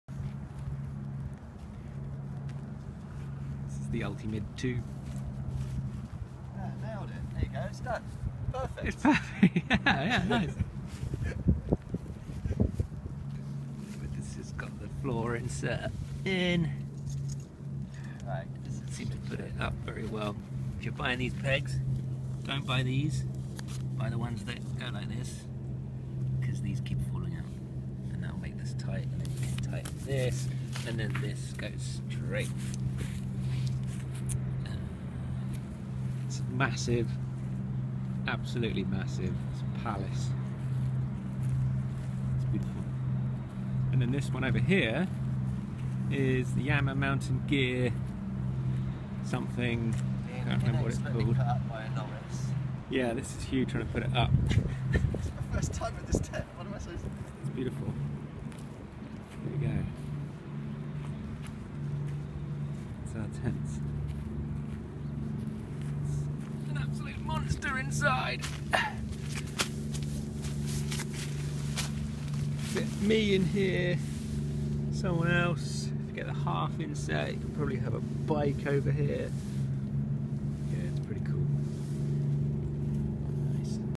Hyperlite Mountain Gear Ultamid 2 pyramid tent
First time setup of my Ultamid 2 alongside my Yama Mountain Gear 1P Cirriform single wall tent..